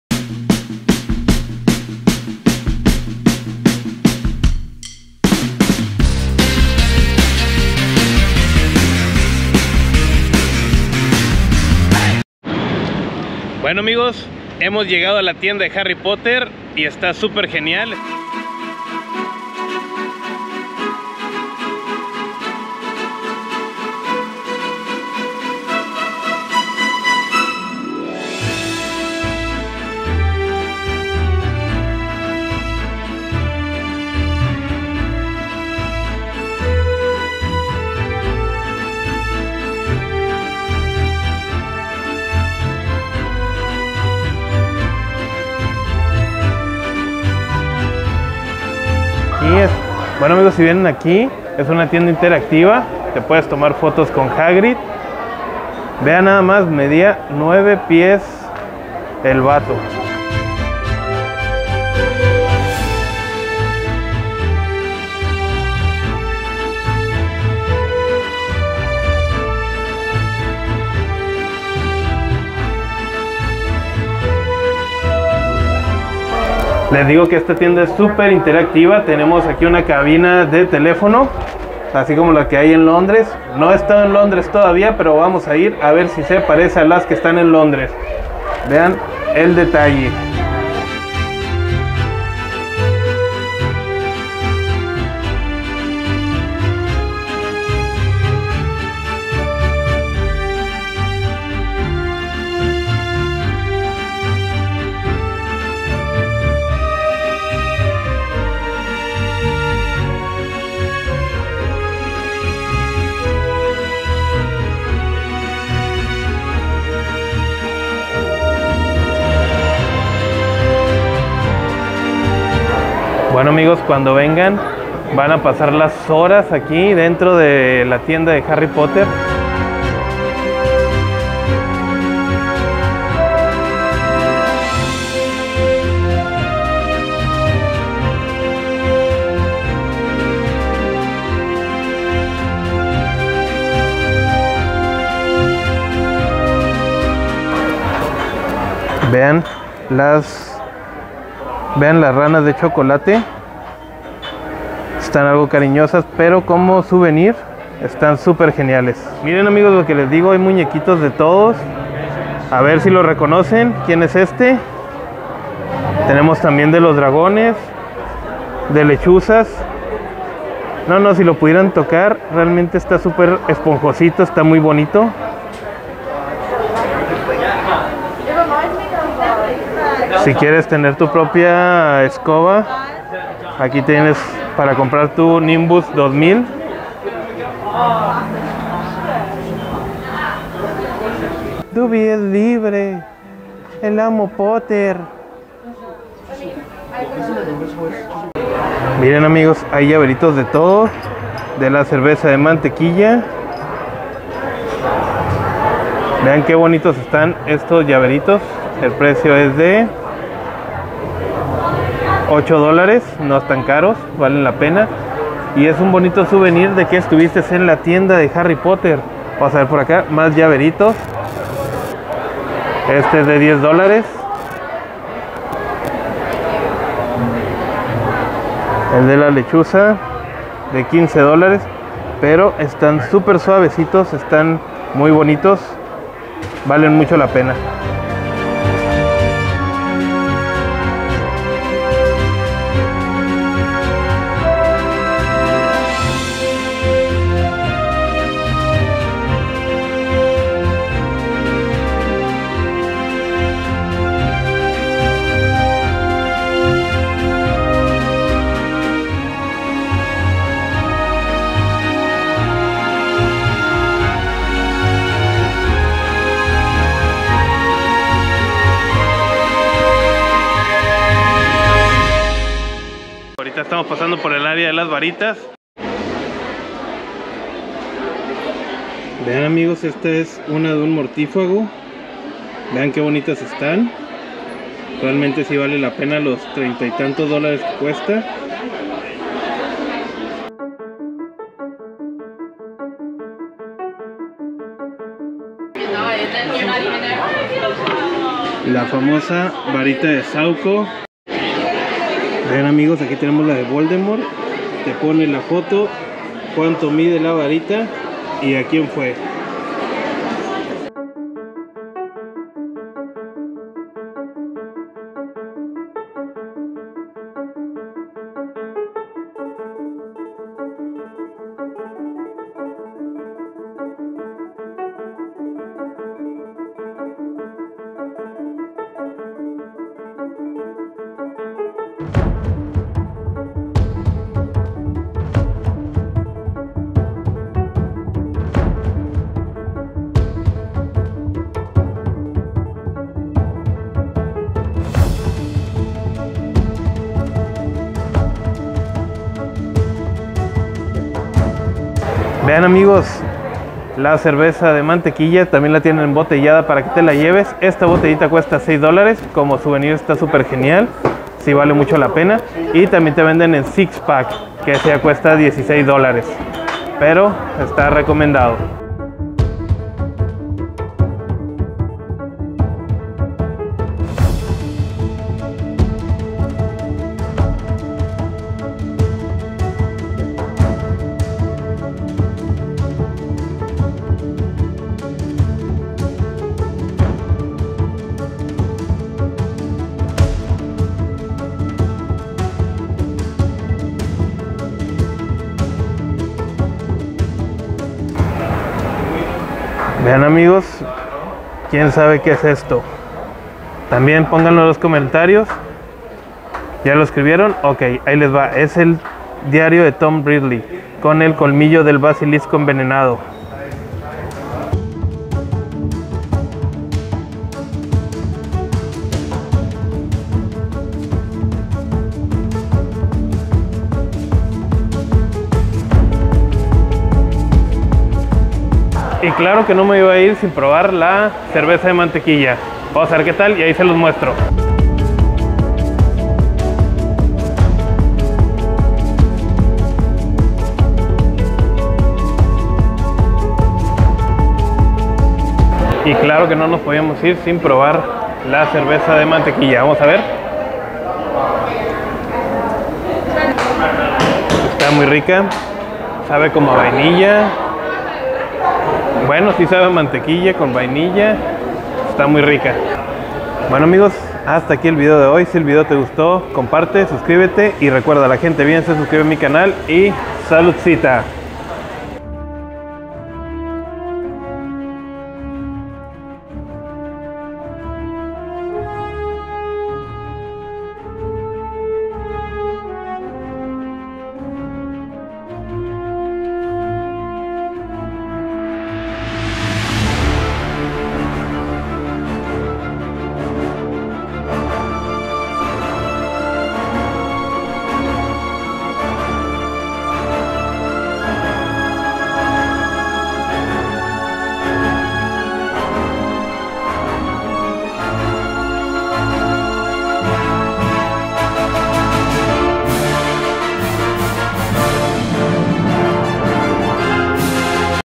Conociendo La Tienda De Harry Potter NY
Estamos conociendo una de las tiendas más nuevas de la franquicia de Harry Potter que se encuentra en Nueva York NY